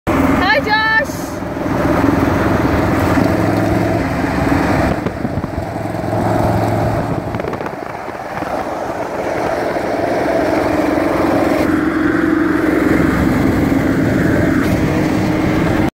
LCIII Spider one-off supercar by Josh Lange
The Lange Customz LCIII Spider driving over the Conejo Grade. The leaded crystal tail lights, reverse lights, front turn signals and daytime running lights are leaded crystal. A one-off aerospace carbon fiber based supercar designed and built by Josh Lange.